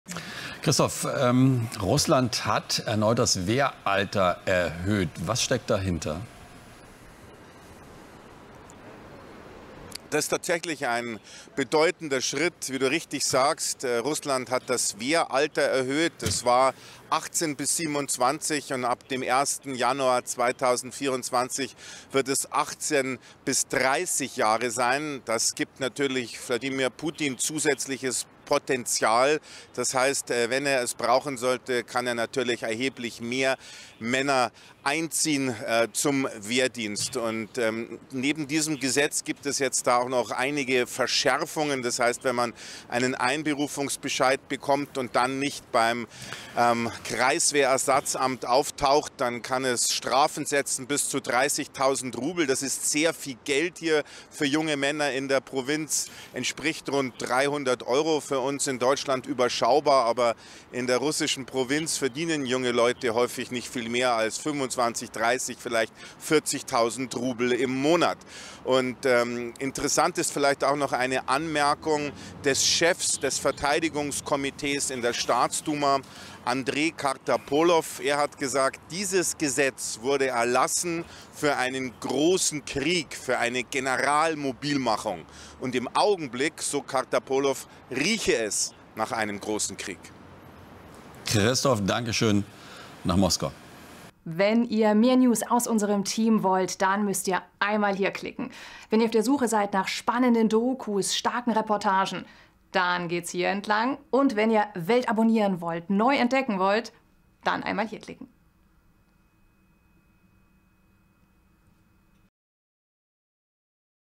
PUTINS KRIEG: "Es riecht nach einem großen Krieg!" Vize-Verteidigungsminister lässt aufhorchen
PUTINS KRIEG: "Es riecht es nach einem großen Krieg!" Vize-Verteidigungsminister lässt aufhorchen

In Russland hat das Unterhaus des Parlaments den Weg für eine Ausweitung des Wehrdienstes freigemacht. Ein am Dienstag von der Staatsduma in zweiter und dritter Lesung verabschiedetes Gesetz sieht vor, dass ab dem 1. Januar 2024 auch Bürger im Alter zwischen 28 und und 30 Jahren für den Militärdienst einberufen werden - zuvor hatte die Altersobergrenze bei 27 gelegen. Durch die Anhebung der Altersgrenze kann Russland auf deutlich mehr Soldaten zurückgreifen, ohne erneut Reservisten mobilisieren zu müssen.

Nach Angaben der Duma verbleibt die Altersuntergrenze für den Militärdienst bei 18 Jahren. Um in Kraft zu treten, muss das Gesetz noch vom Föderationsrat, dem Oberhaus des Parlaments, gebilligt und von Präsident Wladimir Putin unterzeichnet werden. Beide Schritte sind üblicherweise aber Formsache.

Die Duma verabschiedete am Dienstag ein weiteres Gesetz, das die Strafen für Wehrdienstverweigerer verschärft. So soll künftig bis zu 30.000 Rubel (umgerechnet rund 300 Euro) zahlen, wer nach Erhalt des Einberufungsbescheids "ohne triftigen Grund" nicht bei der zuständigen Dienststelle erscheint.

Bereits im April hatte das russische Parlament die Einberufung Wehrpflichtiger erleichtert, indem es die Zustellung der Bescheide auch über ein staatliches Online-Portal ermöglichte. Zudem sah das Gesetz erhebliche Verschärfungen für Menschen vor, die sich der Wehrpflicht entzogen: Ihnen wird seither verweigert, als Unternehmer oder Selbstständiger zu arbeiten - und der Zugang zu Krediten, die Anmeldung einer Wohnung und die Zulassung eines Autos verweigert.

Zahlreiche Russen hatten sich der Einberufung zur Armee bisher entzogen, indem sie nicht mehr an ihren offiziellen Adressen aufzufinden waren oder Rekrutierungsoffizieren oder Ärzten Schmiergeld zahlten.

Trotz der verschärften Regelungen dementiert der Kreml bisher, eine zweite Mobilisierungswelle wie im vergangenen September starten zu wollen. Damals hatte Russland eine Teilmobilmachung von 300.000 Männern angeordnet, um seine im Februar 2022 begonnene Militäroperation in der Ukraine zu stützen. Daraufhin waren zehntausende Russen aus dem Land geflohen, um sich dem Dienst an der Front zu entziehen.

"Dieses Gesetz wurde für einen großen Krieg erlassen", sagt der Chef des Verteidigungskomitees der Duma, Andrei Kartapalow. WELT-Korrespondent Christoph Wanner berichtet aus Moskau.